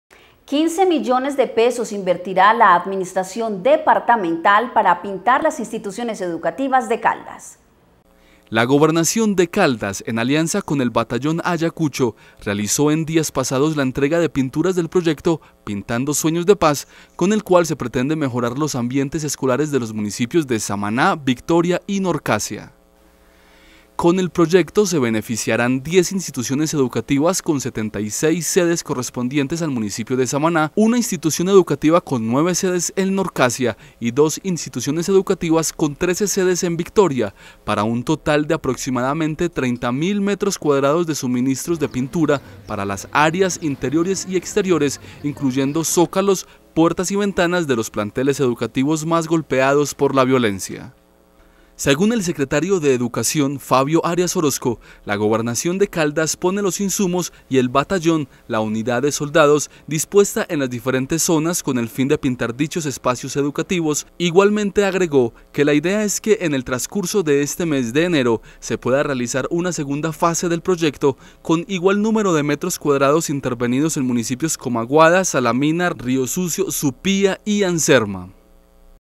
PINTANDO SUEÑOS DE PAZ SE TOMA CALDAS
15 millones de pesos invertirá la administración departamental para pintar instituciones educativas de Caldas.